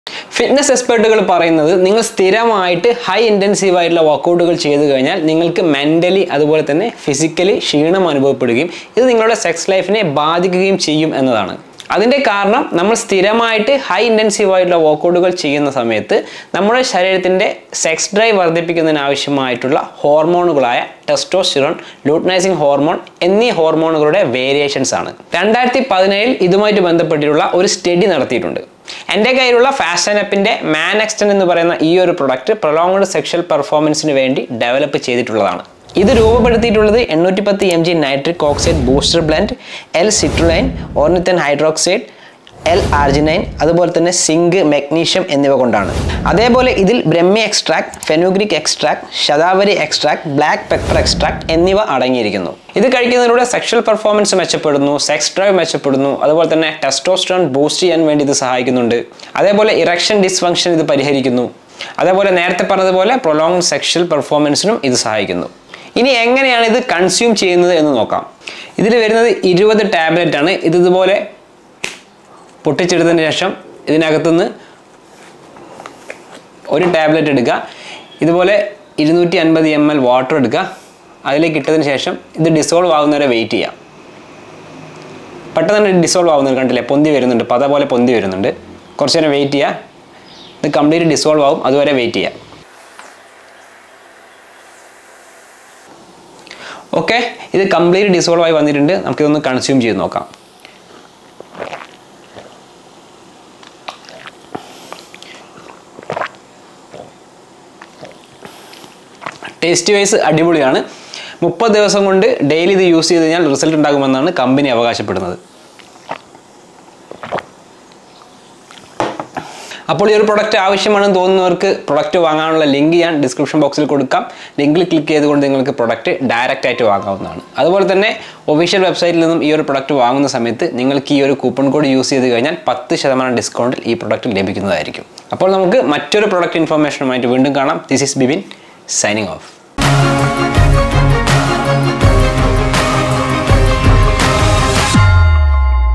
ഒരു കിടിലം സാനം FAST&UP MAN EXTEND - PROLONGED SEXUAL PERFORMANCE | BB FITNESS GUIDE
DESCRIPTION-
Many men face the problem of experiencing unsatisfactory sexual performance due to climaxing early.
Fast&Up Man Extend is specially formulated to delay your climax & extend your performance, allowing
you to have an enhanced sexual experience. It contains 810 mg of Nitric Oxide Booster blend and 65 mg
of Energizing blend that helps elevate the sexual desire and duration of your performance.
@FastandupIn Man Extend is a well-researched nitric oxide boosting supplement that contains a
combination of L-Arginine, L-Citrulline DL Malate and, L- ornithine hydrochloride, that helps stimulate
blood flow to the vital organs to boost vitality, delays fatigue and improve physical performance in men.
It also contains herbal energizing blend that contains black pepper extract, Brahmi extract, fenugreek
extract (Testofen), Shatavari extract that helps you to increase stamina, vitality in men and acts has male
performance enhancer. This nitrate oxide supplement is made with Swiss Effervescent Technology for
faster action and absorption, contains zero sugar, is 100% vegetarian, comes in a Fizzy Peach flavor and is
very gentle on the stomach. We ensure high quality and Fast&Up Man Extend is FSSAI compliant and
Fast&Up Quality Assured. Drop a tablet of this supplement for male performance in 250ml of water, wait
for it to fizz & dissolve completely and can be consumed 30 minutes before the male performance.
Good luck and let's get started!❤️💪
Thanks for visiting.
I wish you good health and life